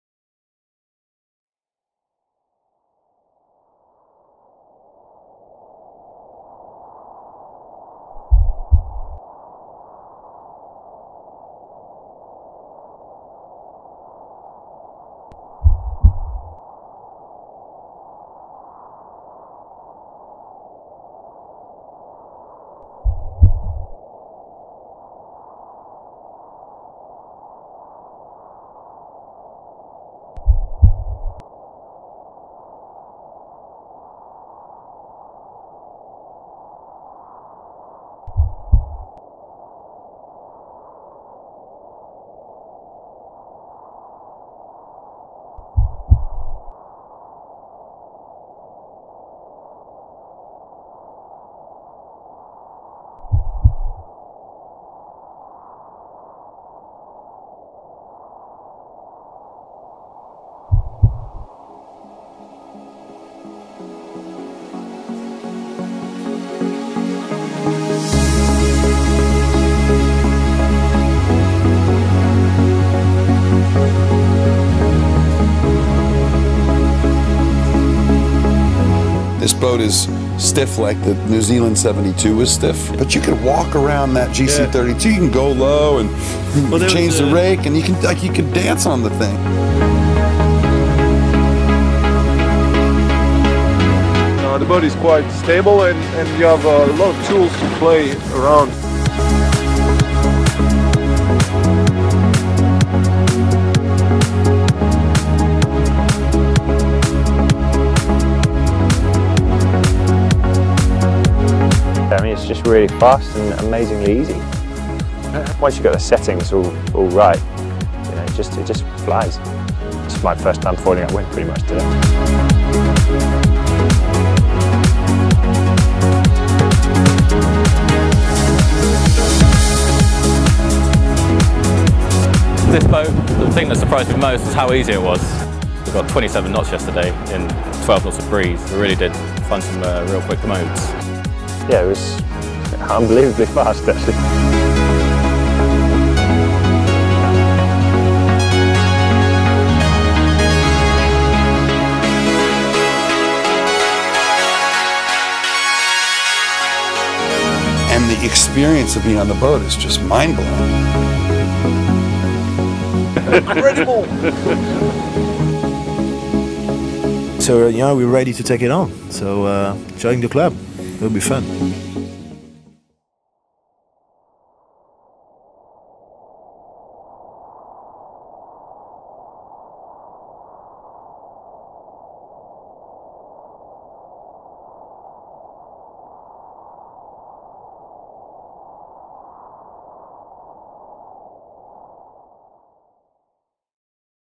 GC32 Promo
Have a look at our new promo video!

We have launched our new GC32 catamaran with its acute L foils and T rudders at La Grande Motte in France. Many came down and have sailed with our one design flying cat. As you can see, they're all very excited. So, join the club. It will be fun! ;-)